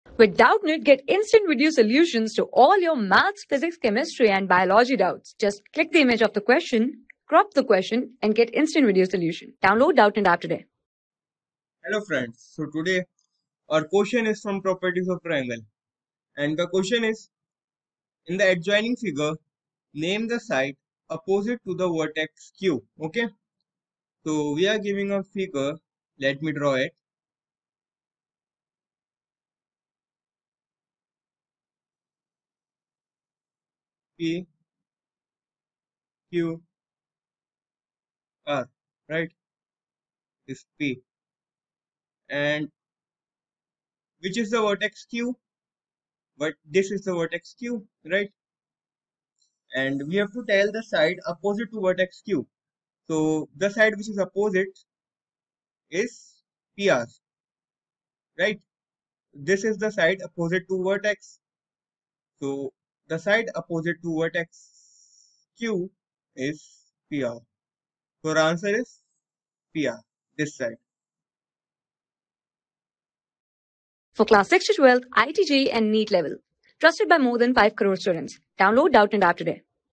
In the adjoining figure : Name the side opposite the vertex Q. | 6 | TRIANGLES | MATHS | ICSE
In the adjoining figure : Name the side opposite the vertex Q.
Class: 6
Subject: MATHS
Chapter: TRIANGLES
Board:ICSE

👉 Have Any Query? Ask Us.

Our Telegram Pages:
class 6
6 class ka math syllabus